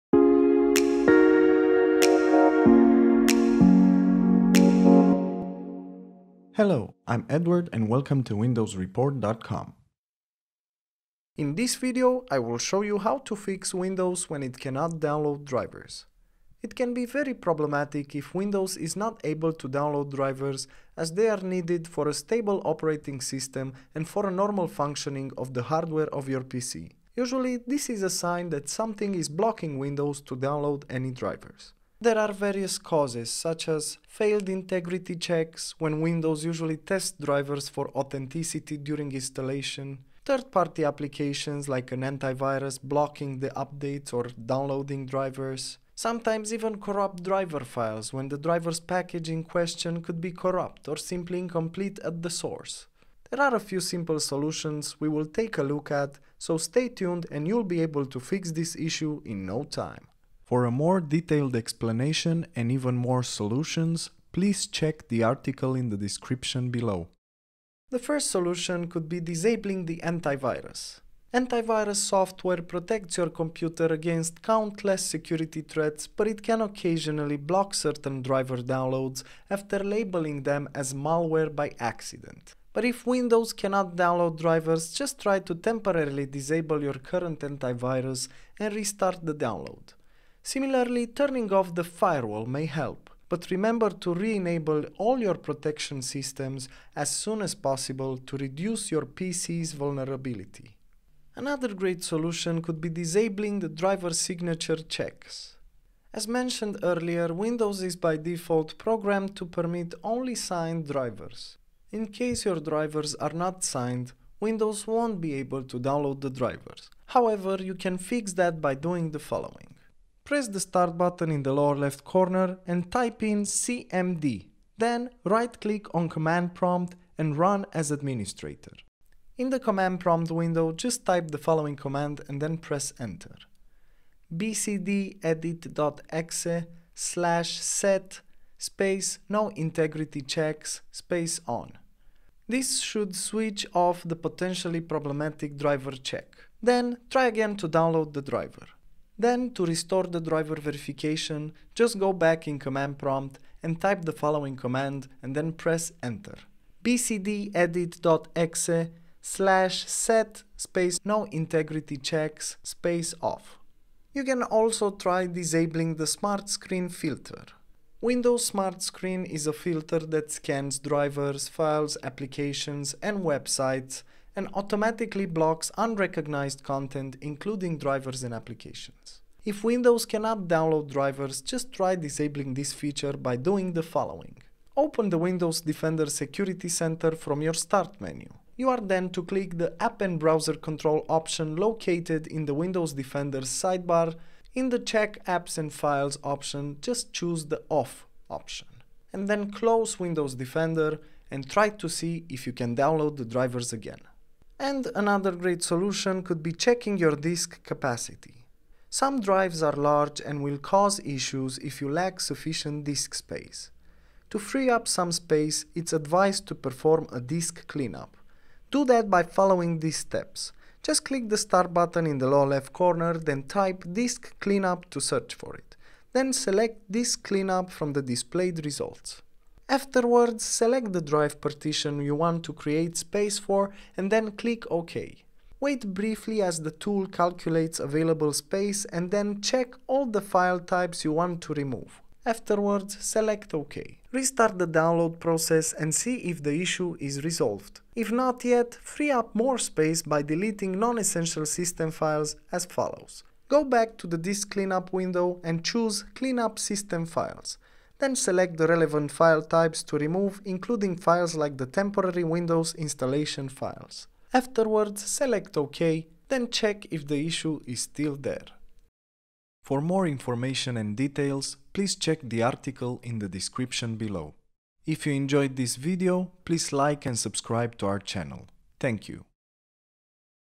Windows cannot download drivers? This is how to fix it
| ⚙️Try Fortect to fix and optimize your PC like a Pro

Snap! Windows cannot download drivers!?
Don't worry too much, just watch this video to see how you can fix it.

0:00 - Intro
1:08 - Disable the antivirus
1:40 - Disable driver signature checks
2:44 - Disable SmartScreen filter
3:25 - Check your disk capacity